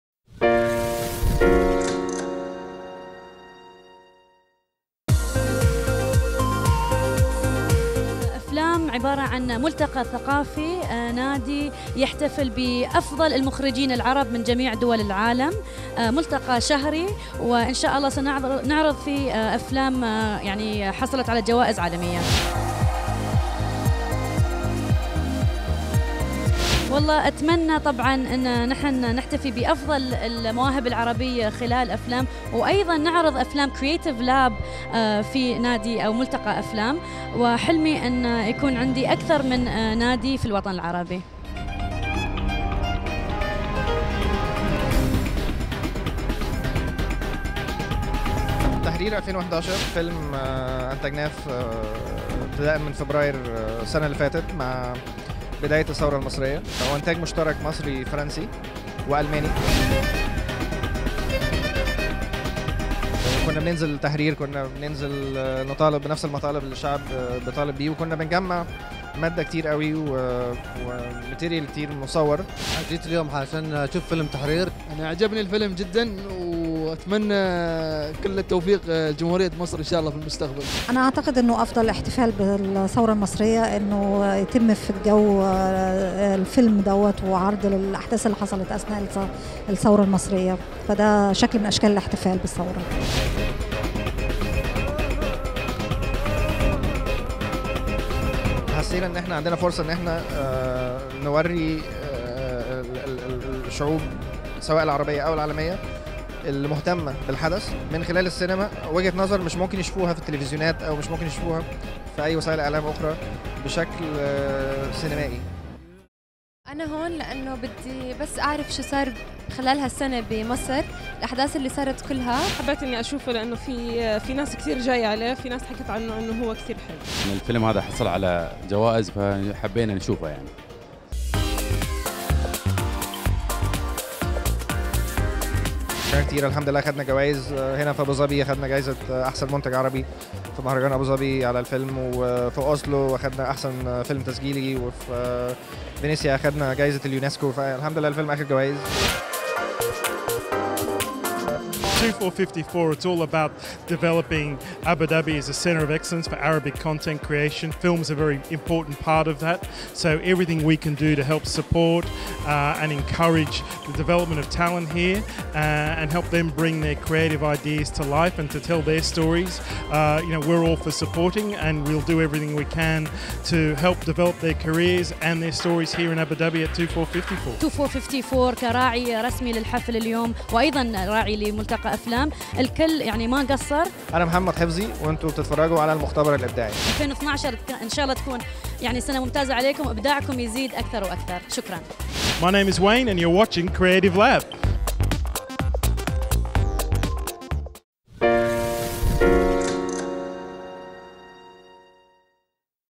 creative lab: aflam launch night
a packed theatre marked the premiere of aflam, nayla al khaja's latest project - a monthly film club in abu dhabi, which launched on 25 january and showcased the award-winning 'tahrir 2011'.